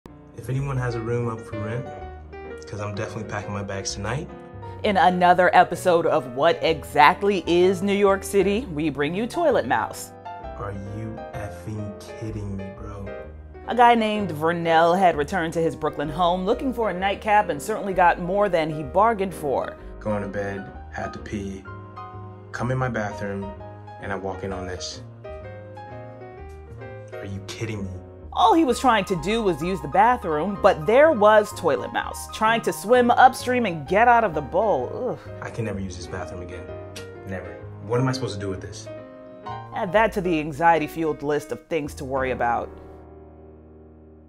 Toilet Mouse Stuns Guy Trying to Use His Bathroom
Vernell returned to his Brooklyn apartment to find a mouse in his toilet when he tried to use it.